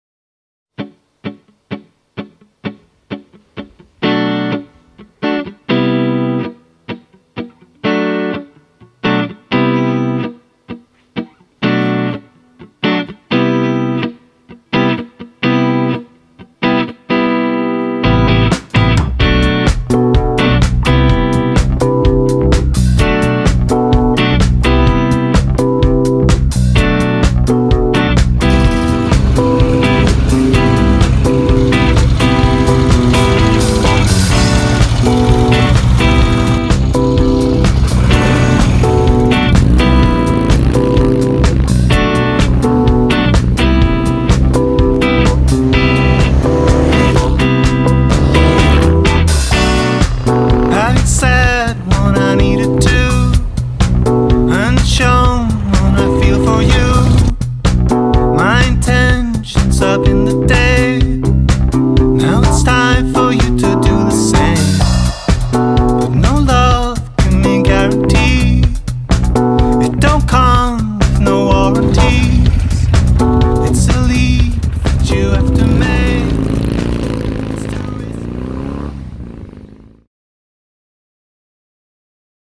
Yamaha XS650 with 750 ccm kit sound and kicking
Small Video from my Testrun with new XS-performance Ignitionsystem.